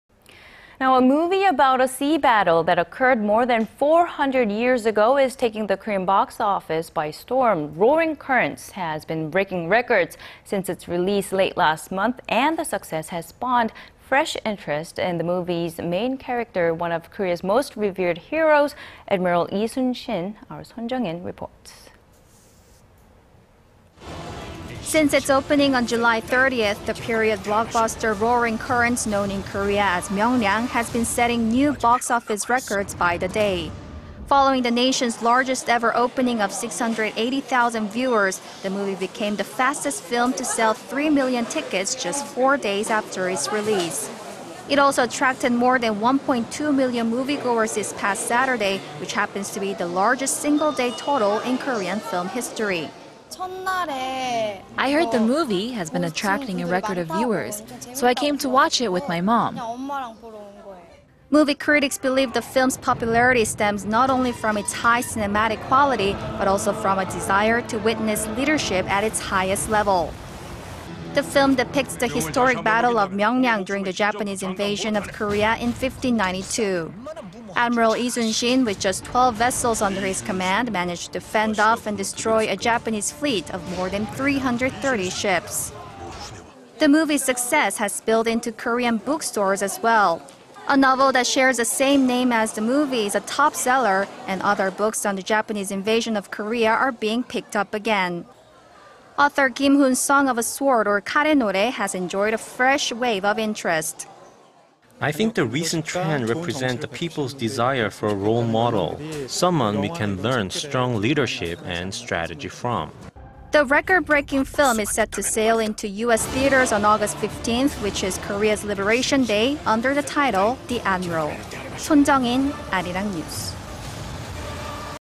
Admiral Yi fever spreads from theaters to bookstores
A movie about a sea battle that occurred more than 400 years ago is taking the Korean box office by storm.
'Roaring Currents' has been breaking records since its release late last month,... and the success has spawned fresh interest in the movie's main character, one of Korea's most revered heroes,... Admiral Yi Sun-shin.
Sohn Jung-in reports.
Since its opening on July 30th, the period blockbuster 'Roaring Currents', known in Korea as 'Myeongryang,' has been setting new box office records by the day.
Following the nation's largest-ever opening of 680-thousand viewers, the movie became the fastest film to sell 3 million tickets, just four days after its release.
It also attracted more than 1-point-2 million moviegoers this past Saturday, which happens to be the largest single day total in Korean film history.

"I heard the movie has been attracting a record of viewers, so I came to watch it with my mom."

Movie critics believe the film's popularity stems not only from its high cinematic quality, but also from a desire to witness leadership at its highest level.
The film depicts the historic Battle of Myeongryang during the Japanese invasion of Korea in 1592.
Admiral Yi Sun-sin, with just 12 vessels under his command, managed to fend off and destroy a Japanese fleet of more than 330 ships.

The movie's success has spilled into Korean bookstores as well.
A novel that shares the same name as the movie is a top seller,... and other books on the Japanese invasion of Korea are being picked up again.
Author Kim Hoon's 'Song of a Sword' or ' ' has enjoyed a fresh wave of interest.

"I think the recent trend represents the people's desire for a role model,... someone we can learn strong leadership and strategy from."

The record-breaking film is set to sail into U.S. theaters on August 15th,... which is Korea's Liberation Day,... under the title, 'The Admiral.'
Sohn Jung-in, Arirang News.